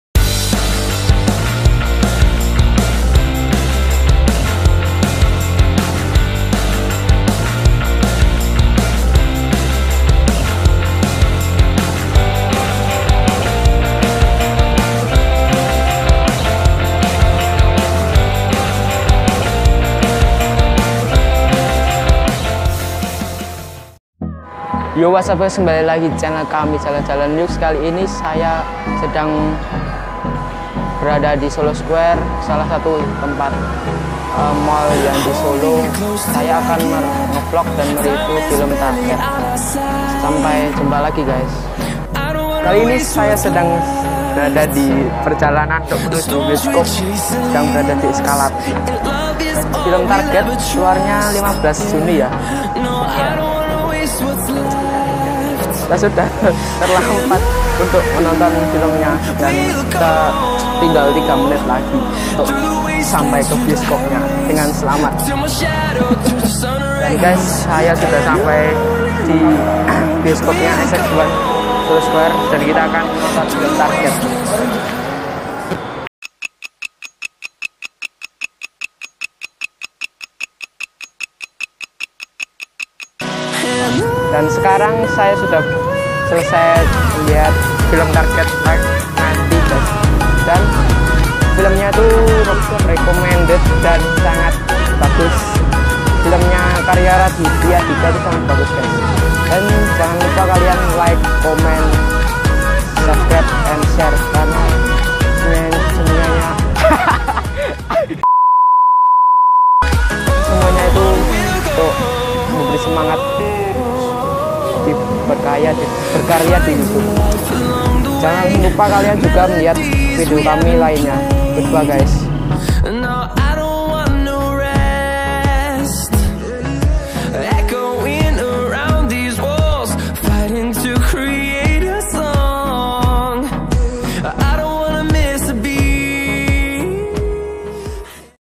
REVIEW FILM TARGET
SINOPSIS :
SEMBILAN ORANG HENDAK SHOOTING FILM BERJUDUL "TARGET",NAMUN MEREKA TERNYATA DIJEBAKDI SEBUAH GEDUNG KOSONG. SESEORANG BERNAMA GAME MASTER MUNCUL, MEMBERITAHU DALAM 24 JAM MEREKA HARUS MENGIKUTI PERMAINAN-PERMAINAN MEMATIKAN. HANYA SATU YANG BISA SELAMAT , SIAPA?

FILM INI COCOK UNTUK DI TONTON DENGAN KELUARGA